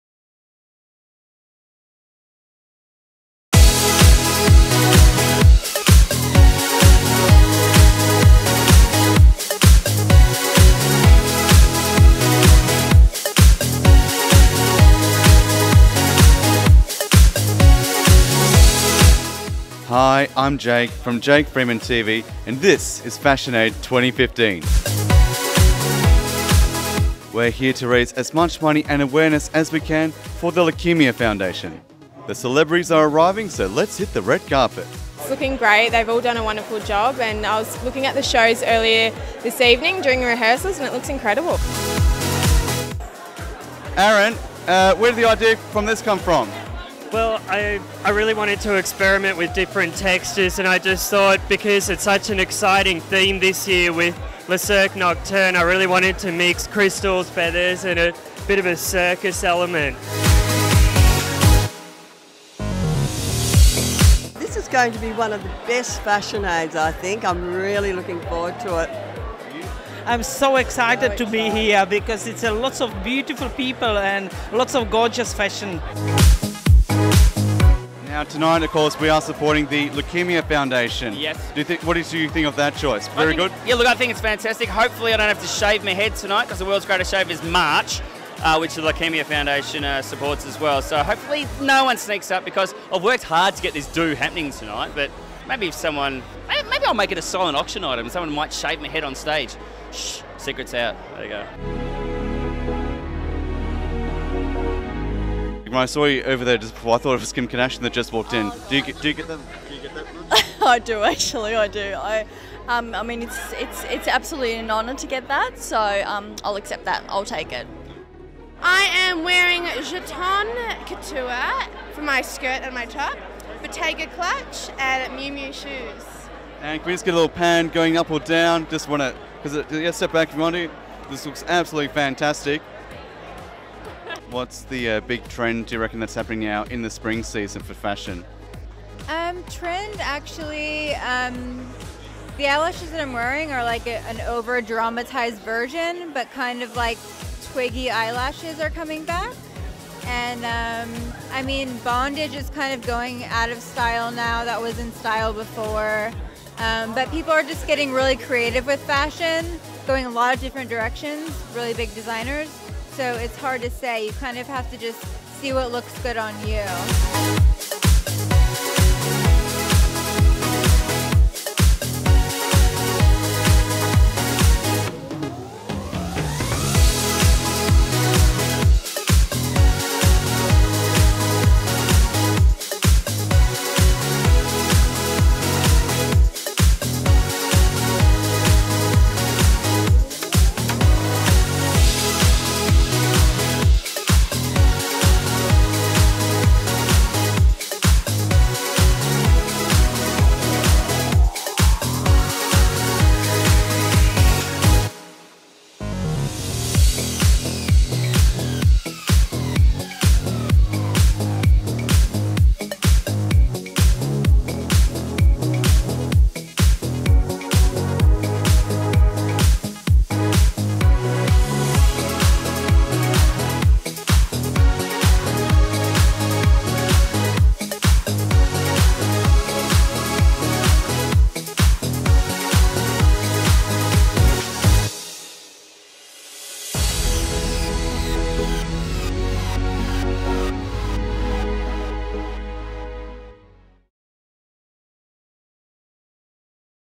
Jake Freeman TV - Fashion Aid 2015
On this edition of Jake Freeman TV, Jake Freeman covers Fashion Aid 2015.

Special thanks to Chris Diprose and Nicholas Murphy from Diprose Media.

Special thanks to Belinda Xerri from Bellephotography.